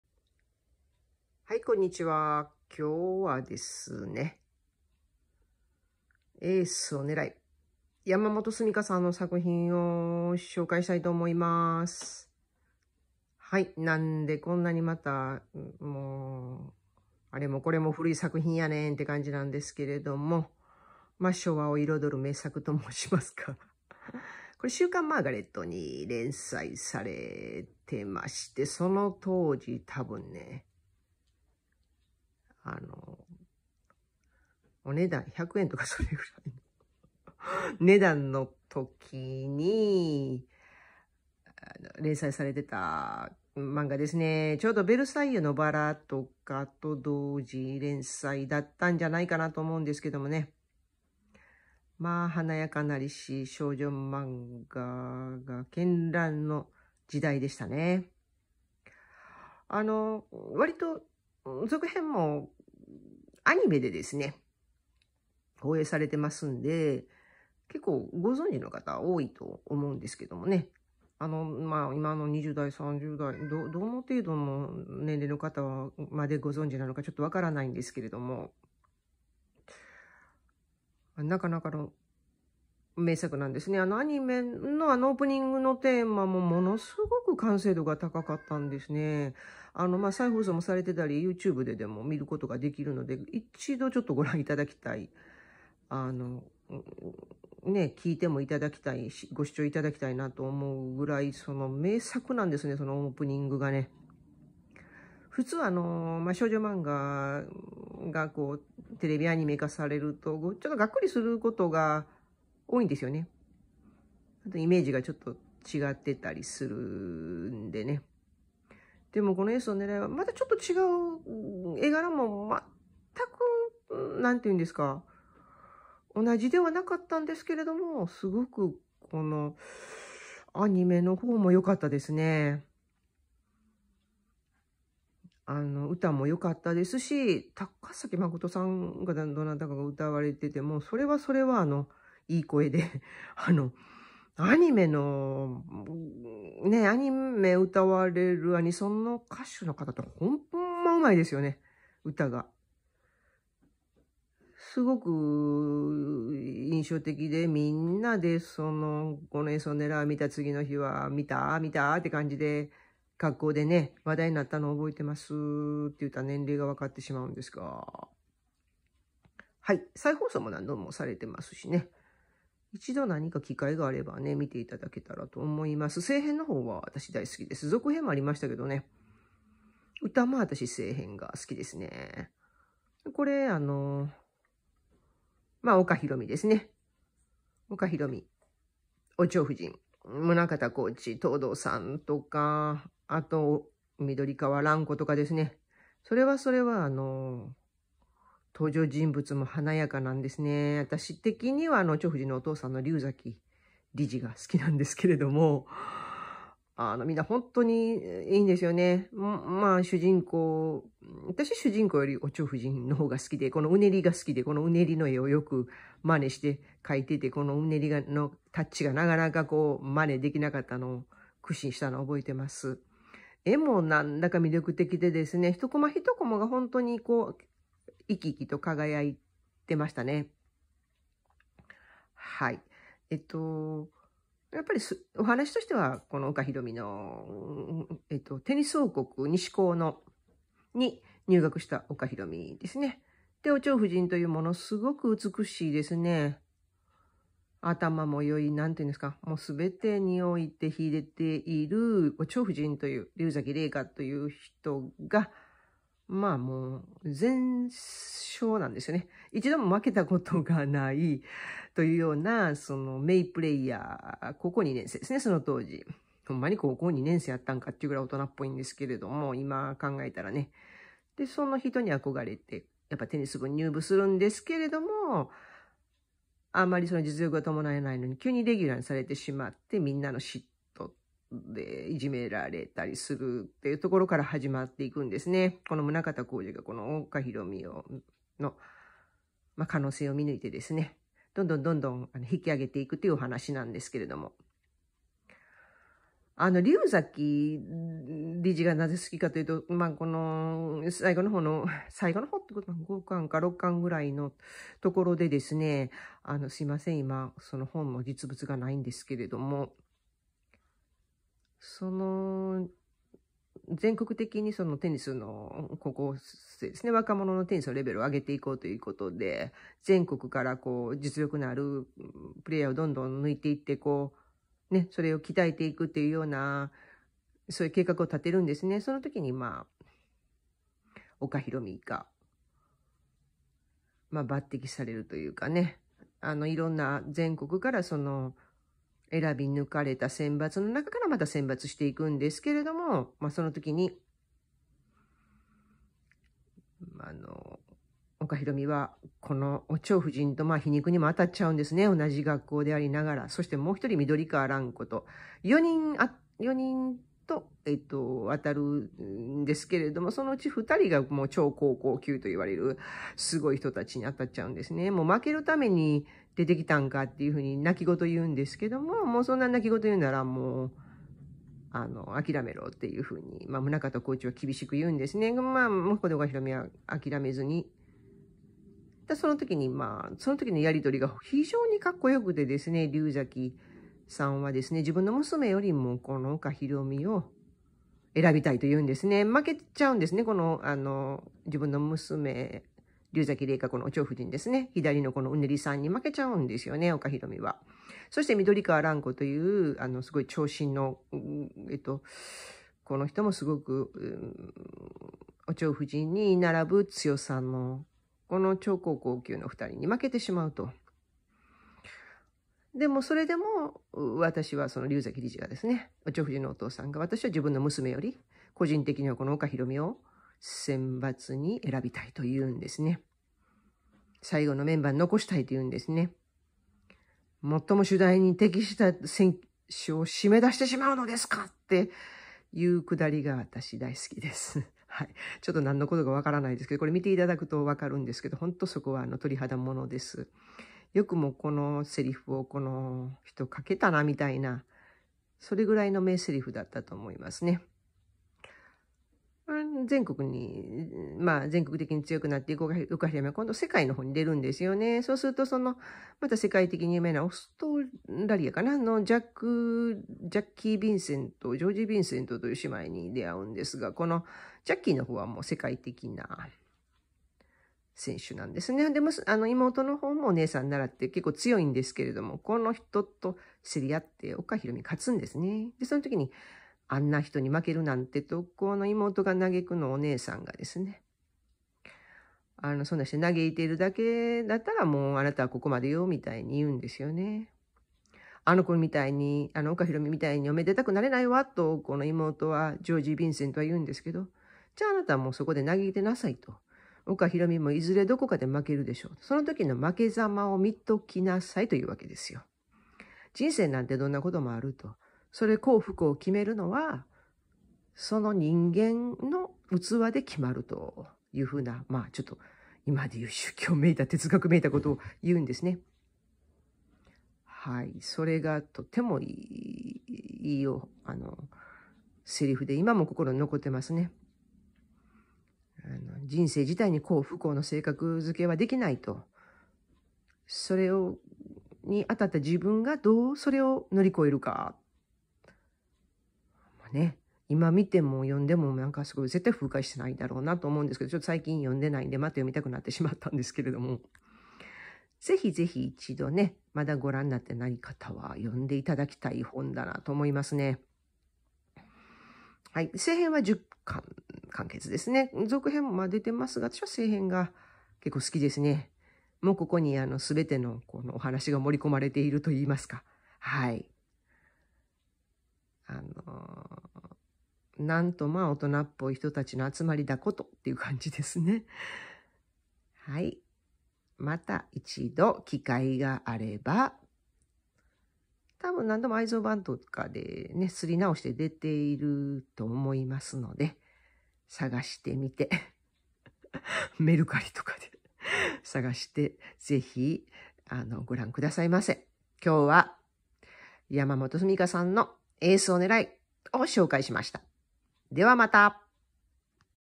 名作少女漫画の部屋 山本鈴美香 エースをねらえ！
すんません、固定してなくて画面ゆらゆらしてます。😅
昭和史に残る少女スポ根の世界。
何度読んでも飽きまへん。

なんてこの時代の人たちは大人っぽかったんだ！

とため息が出るくらいかっちょ良くて、チャーミングな人が目白押しに登場します。
アニメも良いんですよ！是非是非一度読んでみてほしい作品です。